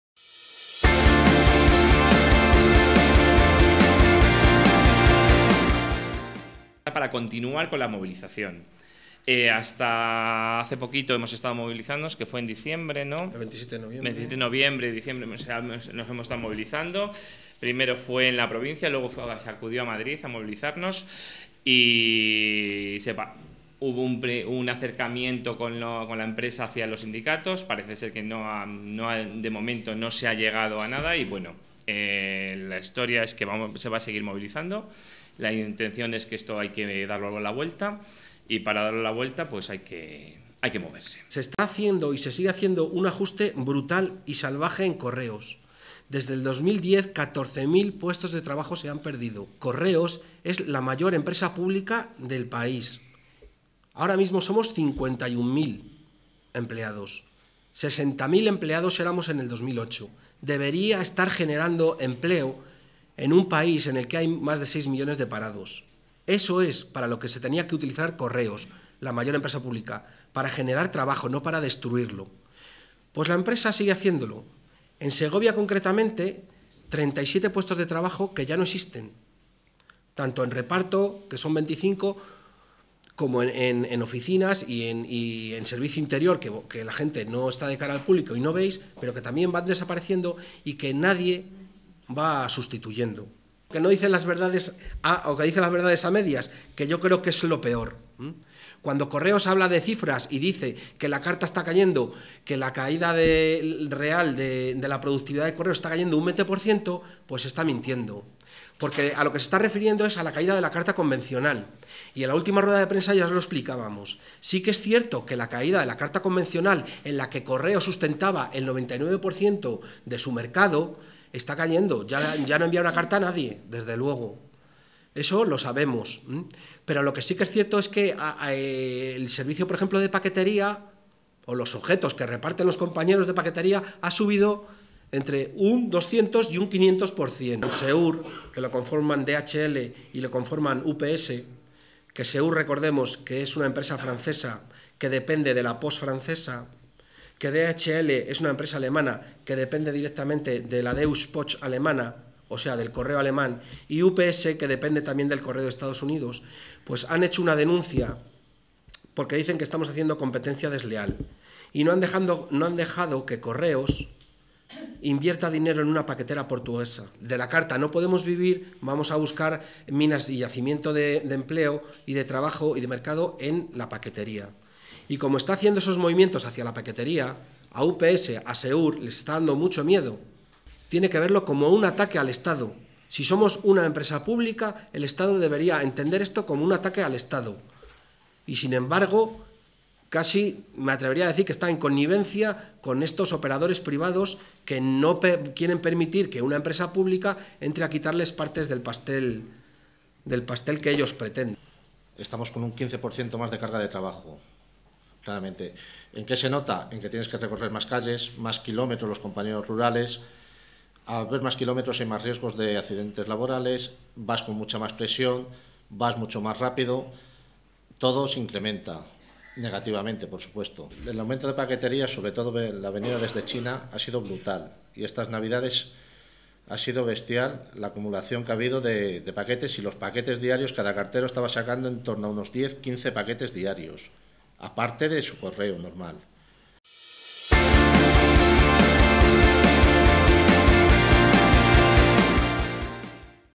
"La paquetería ha subido entre un 200% y un 500%" pero cada vez hay menos trabajadores en Correos
"Se está haciendo un ajuste brutal y salvaje en Correos. Desde 2010, 14.000 puestos de trabajo se han perdido" en la mayor empresa pública del país, que "debería estar generando empleo", ha informado este martes Benito Gómez, responsable de la sección sindical de CCOO de Correos en Segovia.